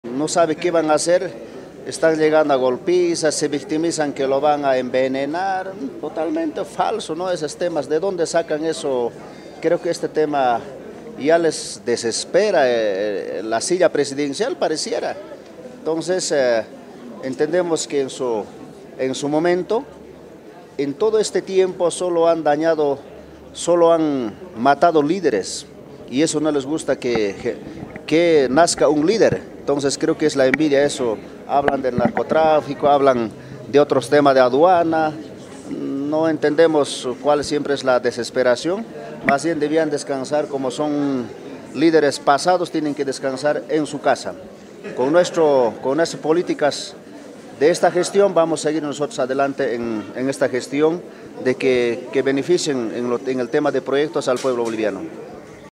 AFIRMAN QUE EL MAS NO ESTARIA ACEPTANDO NUEVOS LIDERAZGOS
DIPUTADO ISRAEL HUAYTARI (MAS) ASEGURA QUE DENTRO DE LA DIRIGENCIA DEL MAS NO PERMITEN LOS NUEVOS LIDERAZGOS Y QUE EN EL PASADO SOLO “MATARON LIDERES“ POR APARENTE ENVIDIA